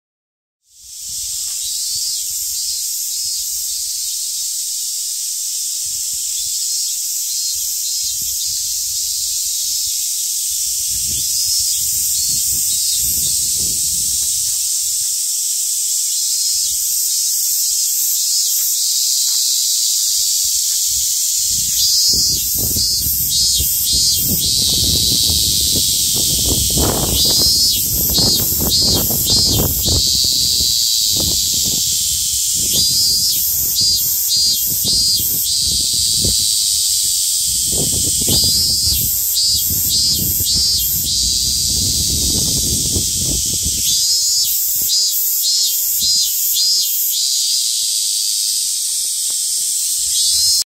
夏2015. Japan summer. Insect called semi coming out.
夏のセミ泣いている。insect called semi coming up and they invite there partner with this sound. Right after that they die.